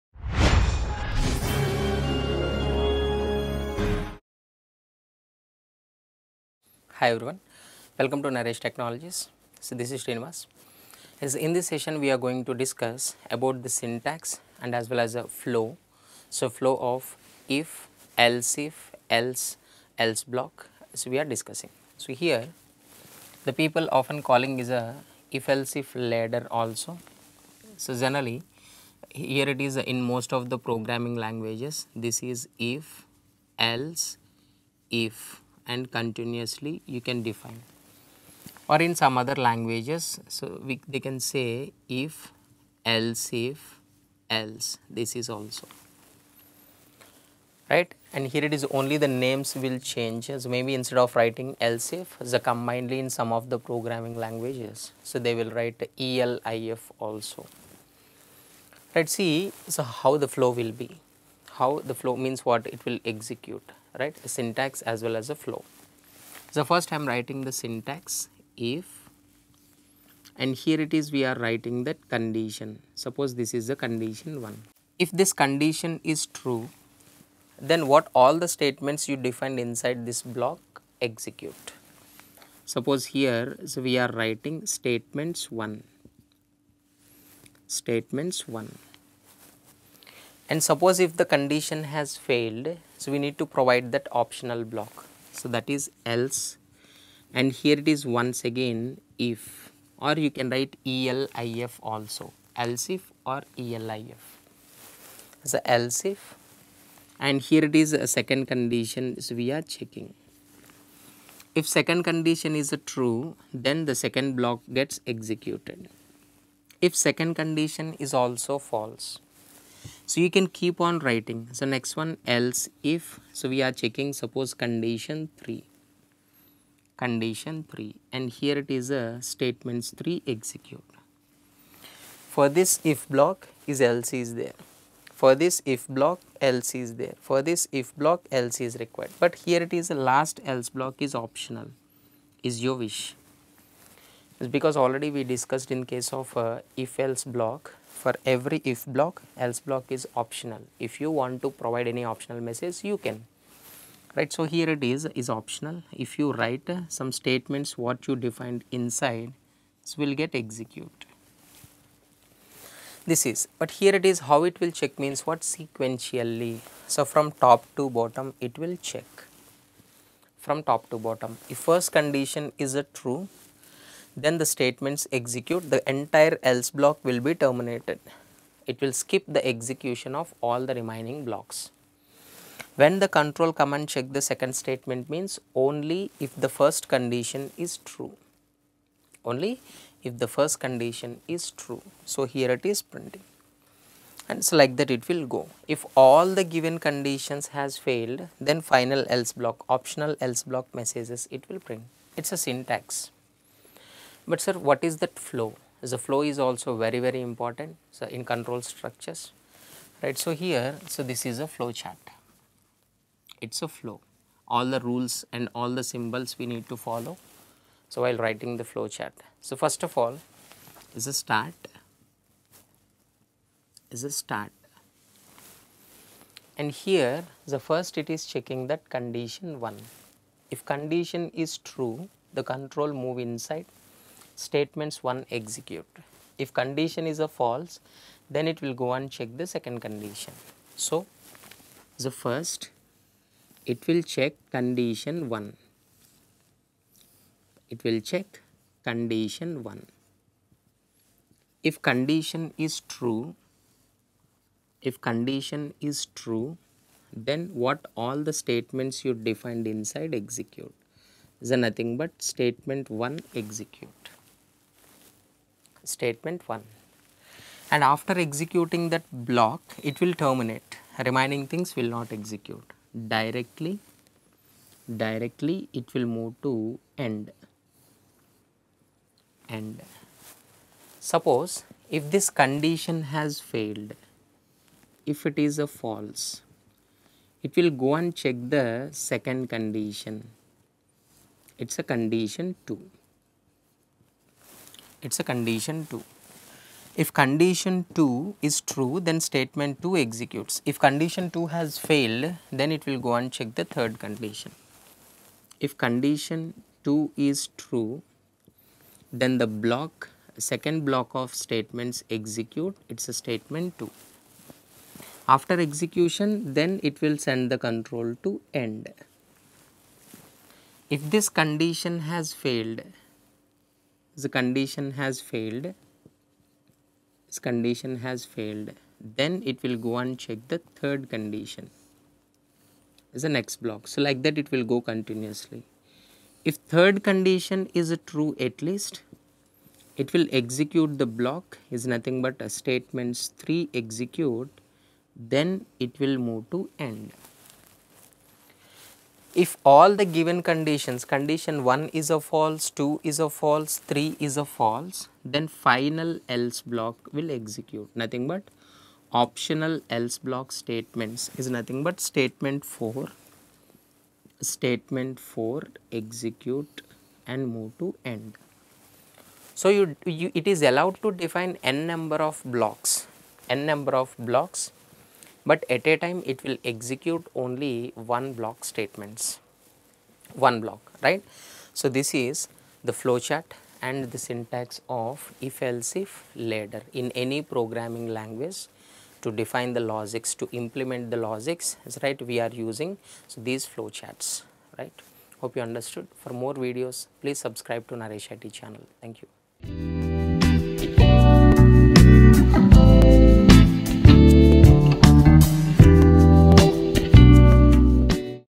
if-else-if block | Control Flow Structures Tutorial | Naresh IT
This video explains clearly syntax and control flow of if else if ladder in logical programming.